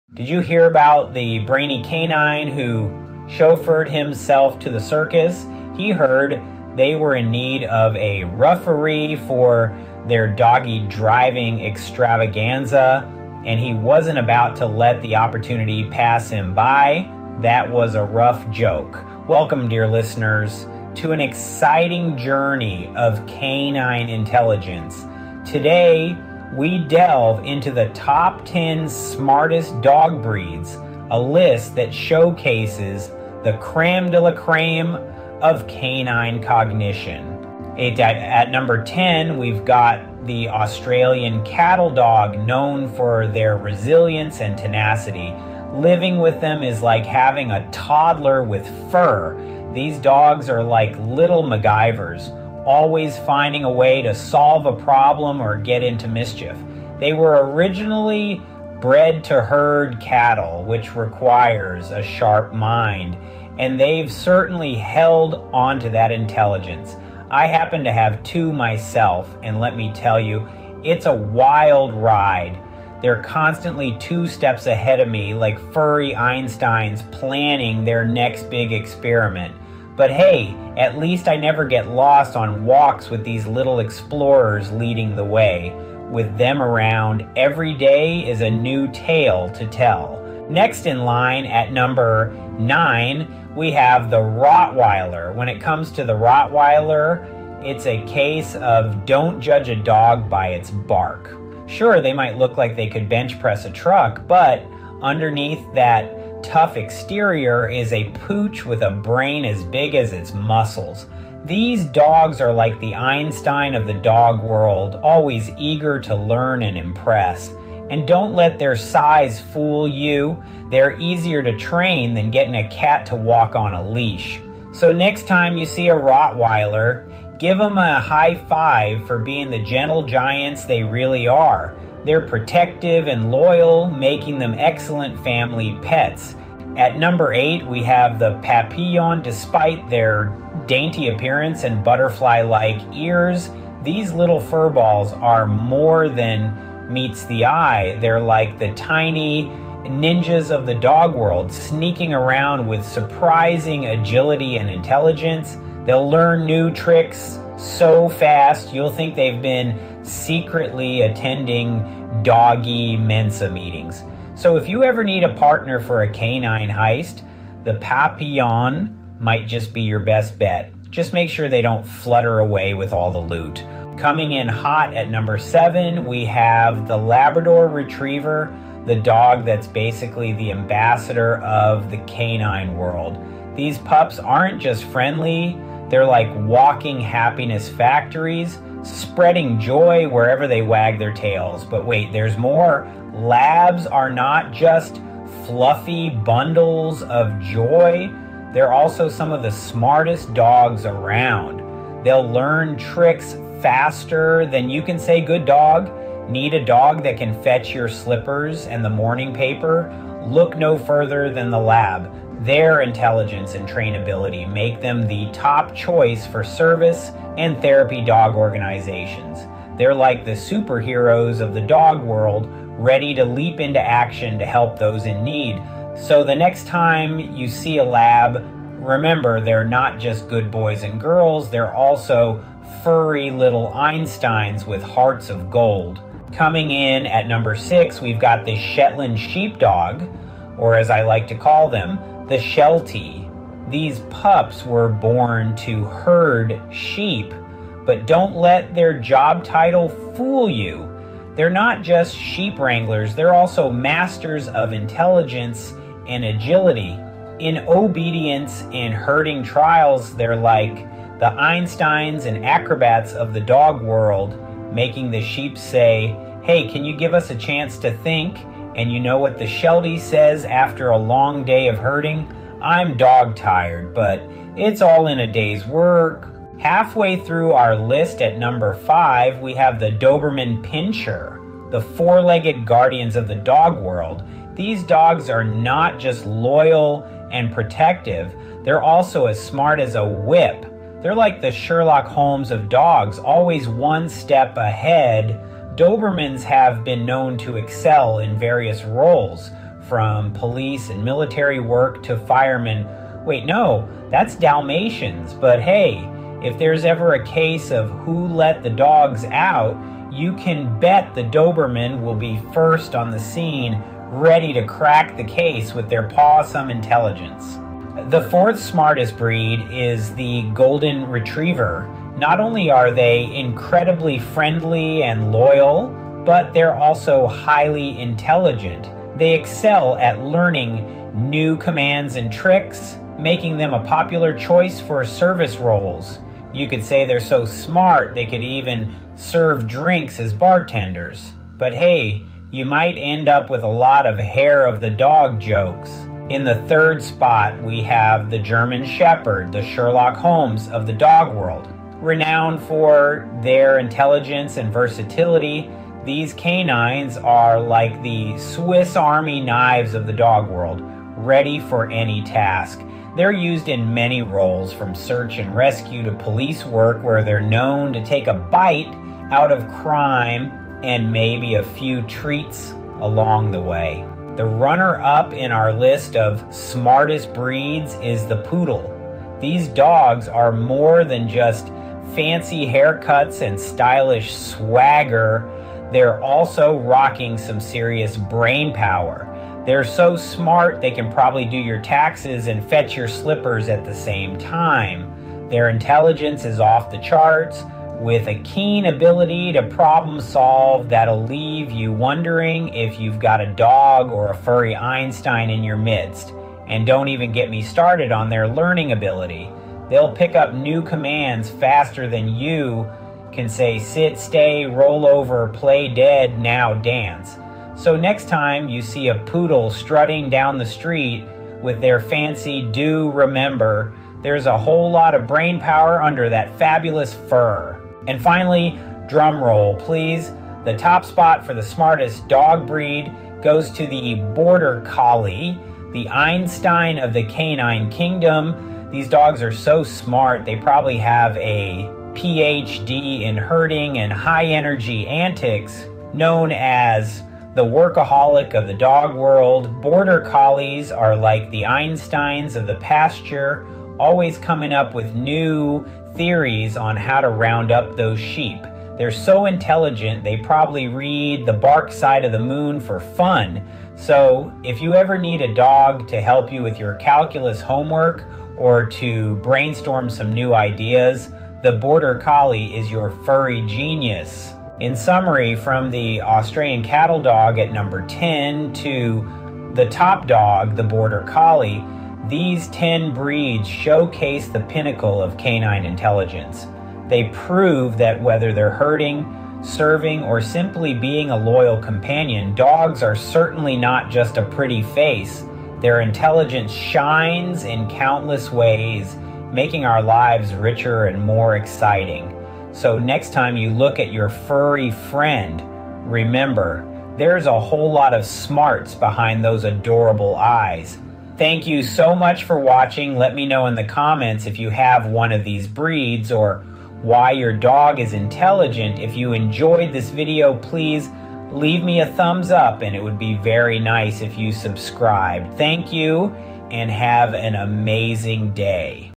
Laugh and Learn: The 10 Smartest Dog Breeds of 2024
Welcome to the ultimate canine comedy show! Join us as we explore the world of the smartest dog breeds, from the clever Border Collie to the problem-solving Australian Cattle Dog. We'll also dive into hilarious circus antics, including a tale of a dog who drove himself to the big top! And of course, get ready for some side-splitting doggy jokes that will leave you howling with laughter. Don't miss this paws-itively funny adventure!